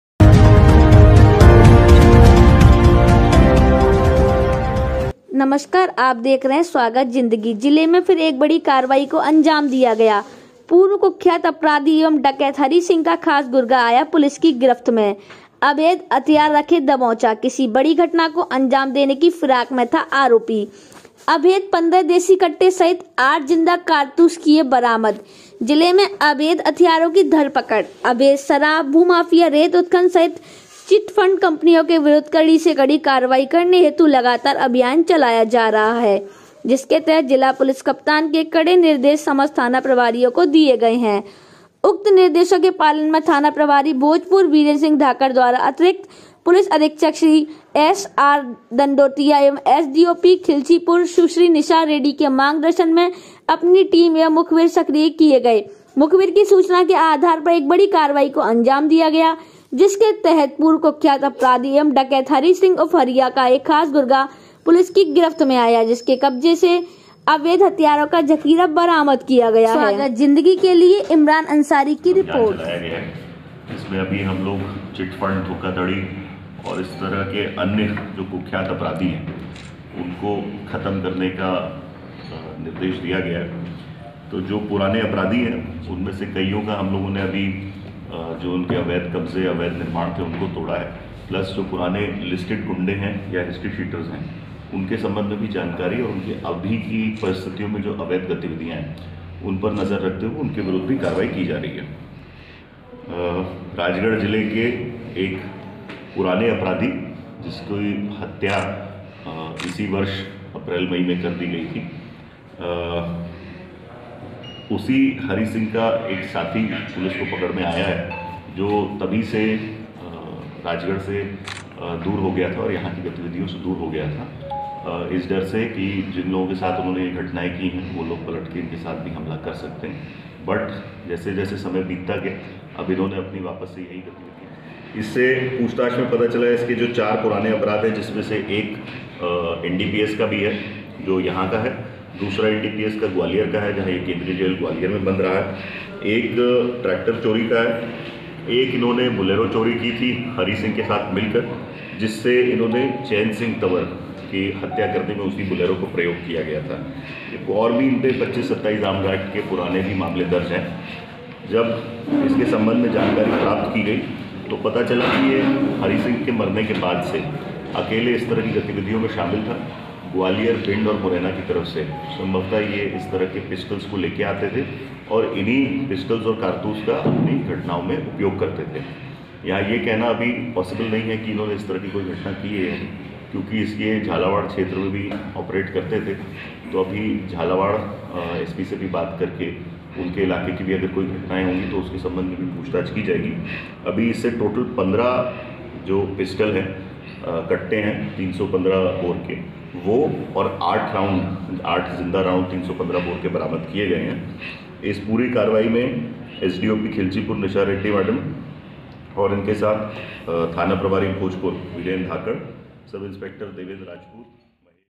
अवैध 15 देसी कट्टे सहित 08 जिंदा कारतूस किए बरामद,अवैध हथियार रखे दबोचा...
थाना भोजपुर, जिला राजगढ़ में अवैध हथियारों की धरपकड़, अवैध शराब,भू माफिया, रेत उत्खनन सहित चिट फंड कंपनियों के विरूध्द कडी से कडी कार्यवाही करने हेतु लगातार अभियान चलाया जा रहा है जिसके तहत जिला पुलिस कप्तान के कड़े निर्देश समस्त थाना प्रभारियों को दिए गए हैं।
             उक्त निर्देशों के पालन में थाना प्रभारी भोजपुर वीरेन्द्रसिंह धाकड द्वारा अतिरिक्त पुलिस अधीक्षक श्री एस.आर.दण्डोतिया एवं एस.डी.ओ.पी. खिलचीपुर सुश्री निशा रेड्डी के मार्गदर्शन में अपनी टीम एवं मुखबिर सक्रिय किए गए, मुखबिर की सूचना के आधार पर एक बड़ी कार्यवाही को अंजाम दिया गया है जिसके तहत पूर्व कुख्यात अपराधी एवं डकैत हरि सिंह उर्फ हरिया का एक खास गुर्गा पुलिस की गिरफ्त में आया है जिसके कब्जे से अवैध हथियारों का जखीरा बरामद किया गया है।
             दिनांक 18 एवं 19/12/2020 को दरमियानी रात्रि में थाना प्रभारी वीरेन्द्रसिंह धाकड को अपने विश्वसनीय मुखविरों के द्वारा सूचना मिली कि थाने का निगरानी बदमाश रामलाल तंवर निवासी बेदर का गंभीर बारदात करने की नियत से अवैध हथियार लेकर इलाके में घूम रहा है, उक्त अपराधी पूर्व डकैत एवं कुख्यात अपराधी हरि सिंह उर्फ हरिया का साथी है जो हरिसिंह उर्फ हरिया के मर्डर का बदला लेने के लिए पूर्व अपराधियों के परिवारजनों को नुकसान पहुंचा सकता है यही नहीं सूचना प्राप्त हुई थी कि बदला लेने की नीयत से रामनारायण तंवर एक गैंग भी बना रहा है इसलिए इतने हथियार बंदोबस्त किए हुए हैं।
             उल्लेखनीय है कि 20 अप्रैल 2020 को हरि सिंह उर्फ हरिया का मर्डर हुआ था तत्समय पुलिस ने तत्पर कार्रवाई करते हुए मर्डर के आरोपियों को गिरफ्तार कर जेल पहुंचा दिया गया था परंतु फिर भी रामनारायण तवर क्षेत्र में असलहा लेकर घूम रहा है जिसे अभी नहीं पकड़ा गया तो निश्चित ही कोई गंभीर बारदात घटित कर देगा।
             सूचना की गंभीरता एवं जनसामान्य की सुरक्षा की दृष्टि से सूचना के बारे में तत्काल वरिष्ठ अधिकारियों को अवगत कराया जाकर टीम गठित की गई ।
           सूचना की तस्दीक हेतू थाना प्रभारी उनि बीरेन्द्र धाकड द्वारा उनि देवेन्द्रसिंह राजपूत चौकी प्रभारी पपडेल को टीम का प्रभारी बनाकर हमराह फोर्स के सूचना की तस्दीक हेतू रवाना किया गया ।
              मुखबिर द्वारा प्राप्त सूचना पर टीम के साथ उक्त बदमाश की तलाश में इलाके में रवाना हुए जिसे नाईहेडा जोड के पास होडामाता मंदिर रोड पर अवैध हथियार एक देशी कट्टा 315 बोर का लोडेड एवं 2 जिंदा कारतूस के हमराह फोर्स की मदद से घेराबदी कर पकडा जिससे हिकमत अमली से पूछताछ करने पर उसने अपने घर ग्राम बेदर में रखा अवैध हथियारों का जखीरा 14 देशी कट्टे एवं 5 जिंदा कारतूस बरामद कराए।
               आरोपी से बरामद उक्त हथियारों के बारे में पूछताछ करने पर उसने एक बड़ा खुलासा किया, आरोपी ने बताया कि ये हथियार हरिया के द्वारा उसे बेचने के उद्देश्य से दिए थे उक्त संबंध में भी आरोपी से पूछताछ की जा रही है।
              उक्त आऱोपी थाने का निगरानी बदमाश है जिसका पूर्व आपराधिक रिकार्ड अपराध क्रमांक 129/2017 धारा 8/15 एनडीपीएस एक्ट, 25,27 आर्म्स एक्ट-आऱोपी अपने साथी के साथ 8.500 किग्रा डोडाचूरा एवं एक लोडेड पिस्टल के साथ पकडा गया था
           2.अपराध क्रमांक 95/2018 धारा 379 भादवि – आऱोपी ने अपने साथियों के साथ एक मेसी ट्रेक्टर चोरी के मामले में पकडा गया था,
          3.अपराध क्रमांक 153/2018 धारा 379 भादवि – आरोपी अपने साथी हरिया उर्फ हरिसिंह तवंर के साथ एक चोरी की बोलेरो गाडी के साथ पकडा गया था एवं उसी गाडी से थाना कोतवाली क्षैत्र में चैनसिंह तवंर निवासी ग्राम माचलपुर की गोली मारकर हत्या की गई थी । आरोपी रामलाल ग्वालियर में अवैध मादक पदार्थ के साथ पकडा गया था जो केन्द्रीय जेल ग्वालियर में भी निरूध्द रहा है ।
           उक्त कृत्य पर आऱोपी रामलाल तवंर निवासी ग्राम बेदर के विरूध्द थाना भोजपुर पर अपराध क्रमांक 375/2020 धारा 25,27 आर्म्स एक्ट का पंजीबध्द कर विवेचना में लिया गया। आरोपी रामलाल से उक्त हथियारों के जखीरे के बारे में पूछताछ की जा रही है आऱोपी को माननीय न्यायालय पेश कि